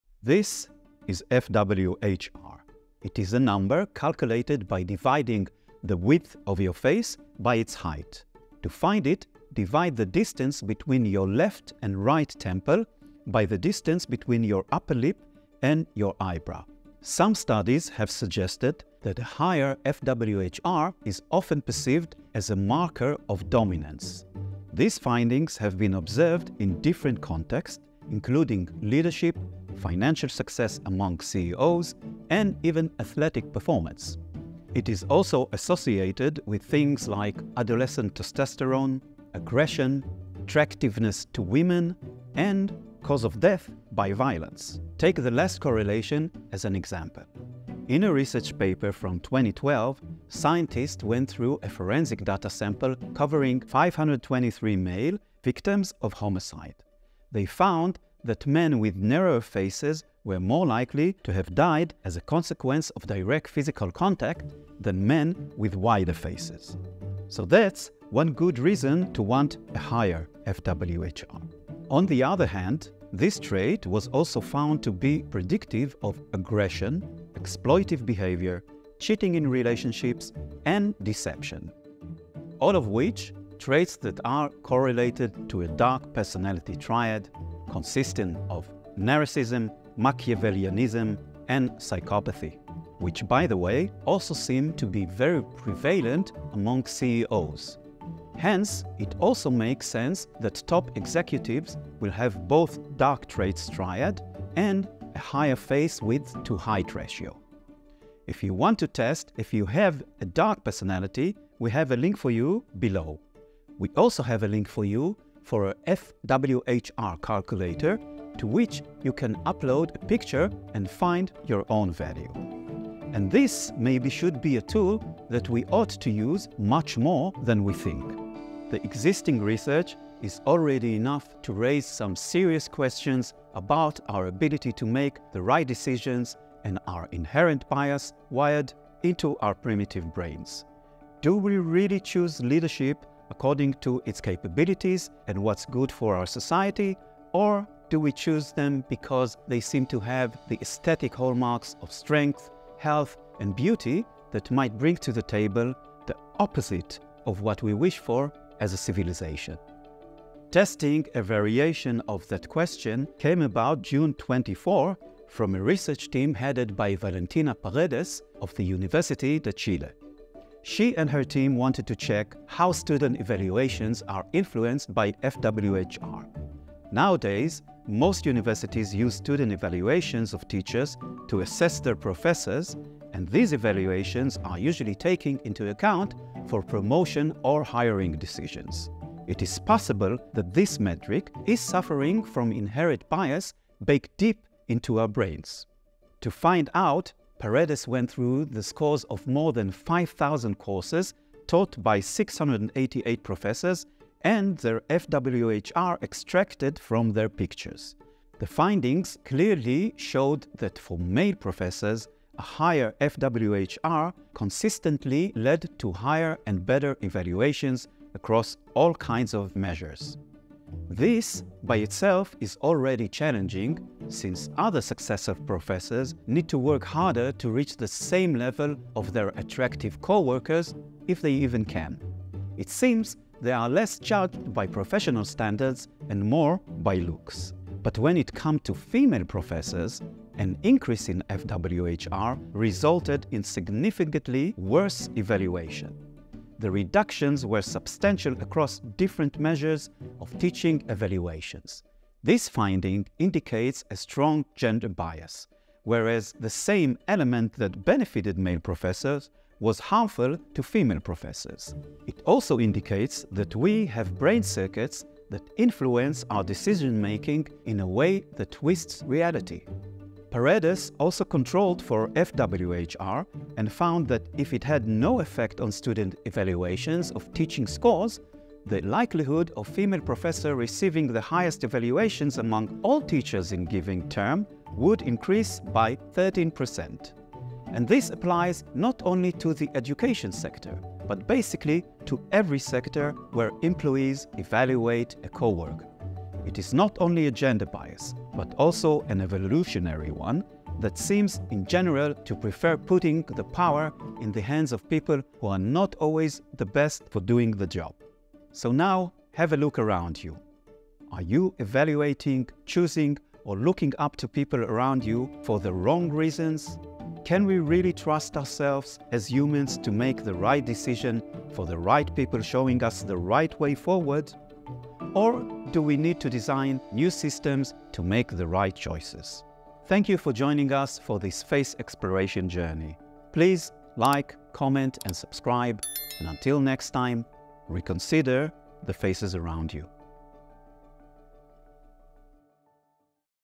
Face the Truth: How Your face porportions Shapes Perception, Power, and Bias
**Unlocking the Secrets Behind fWHR: Dominance, Leadership, and Bias** 🌟

Ever wondered what your facial width-to-height ratio (fWHR) says about you? 🤔 Dive into the fascinating world of fWHR, a metric that reveals much more than just facial proportions.

1. **The Science of fWHR:** Learn how to calculate your fWHR and what it means.
2. **Dominance and Leadership:** Discover why a higher fWHR is often perceived as a marker of dominance, linked to leadership, financial success, and athletic prowess.
3. **The Dark Side:** Uncover the darker traits associated with a higher fWHR, including aggression, deception, and the infamous dark triad of personality—narcissism, Machiavellianism, and psychopathy.
4. **Gender Bias in Evaluations:** See how fWHR affects evaluations differently for male and female professors, highlighting inherent biases in our decision-making processes.
5. **Evolutionary and Societal Implications:** Question whether our inherent biases impact our choices of leaders and decision-makers in society.

**Research Highlights:**
- A forensic study on homicide victims and the correlation with fWHR.
- A comprehensive analysis of student evaluations and fWHR by Valentina Paredes and her team at the Universidad de Chile.

Are we judging people for the wrong reasons? Should we rethink how we choose our leaders and influencers?

Until next time, keep reconsidering the faces around you!

*Watch the video now and challenge your perceptions!*